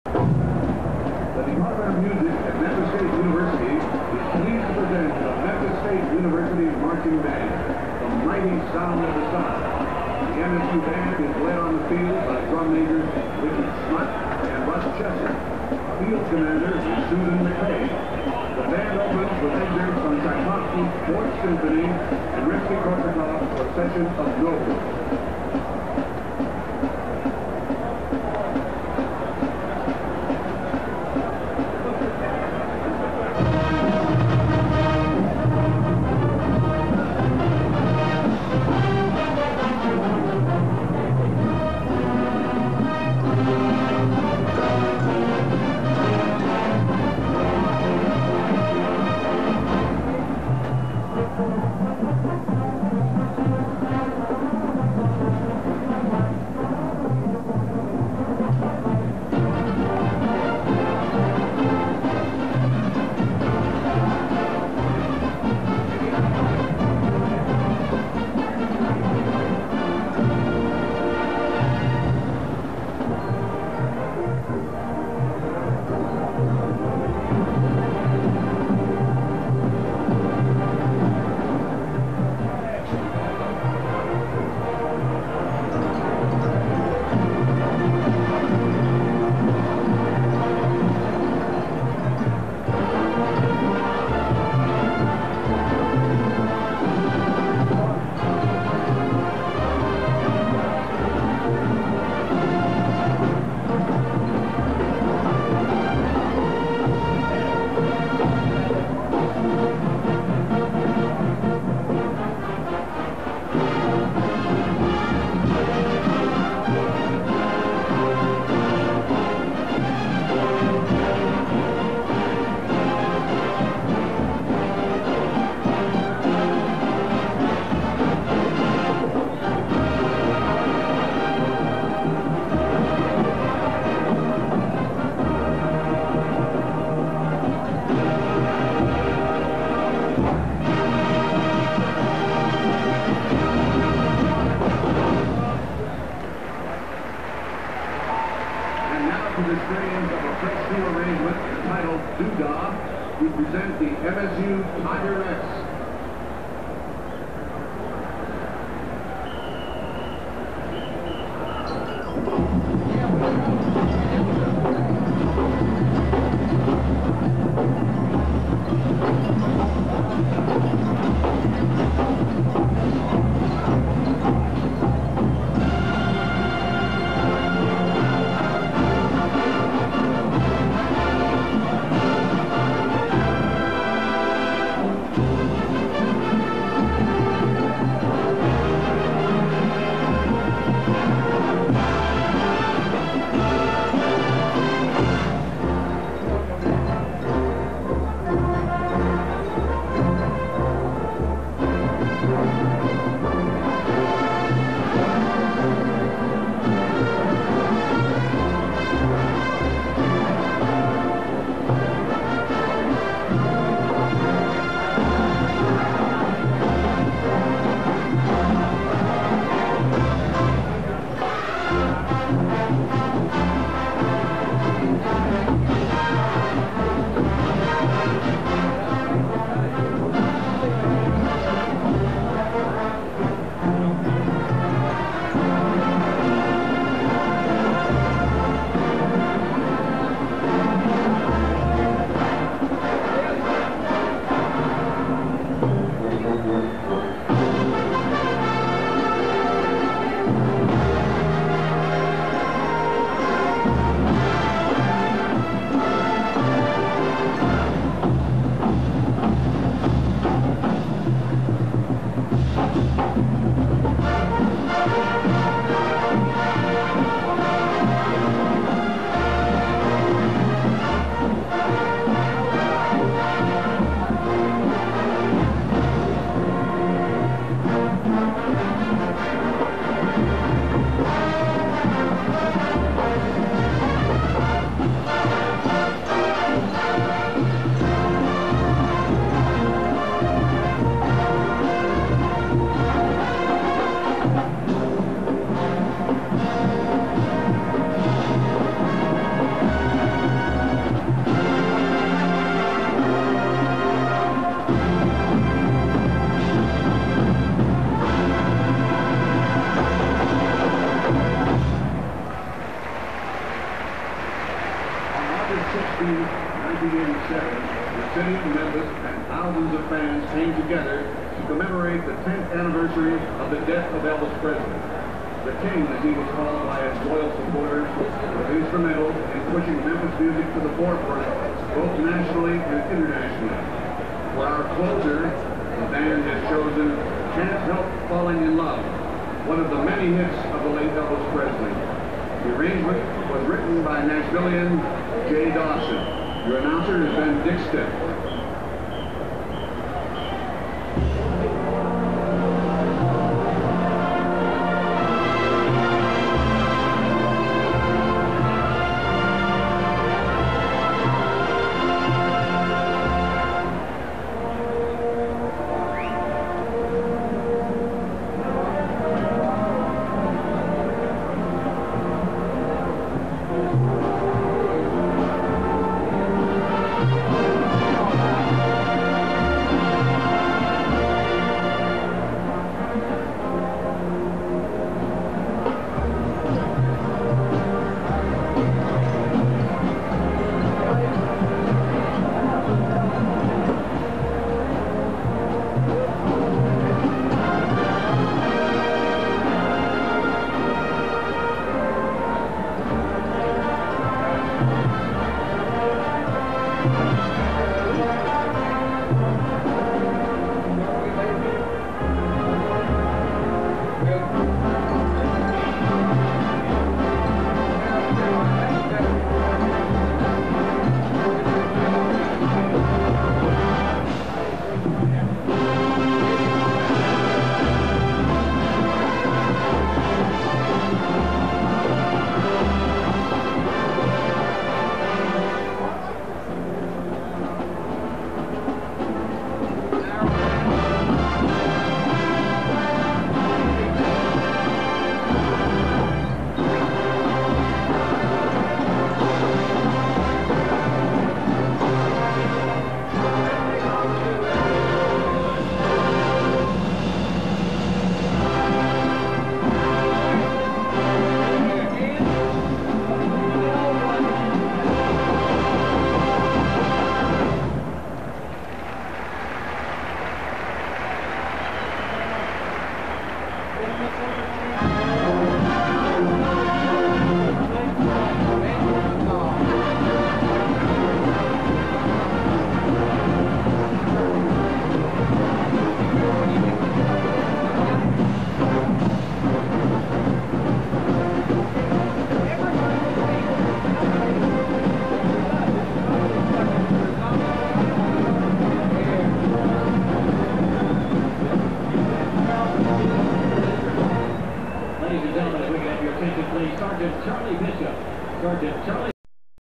Memphis State University Marching Band - 1987 (Halftime at Vanderbilt)
Halftime performance of the Mighty Sound of the South at the September 12, 1987 game with Vanderbilt at Vanderbilt Stadium in Nashville.

Converted from Betamax videotape.  Presented by the University of Memphis Band Alumni.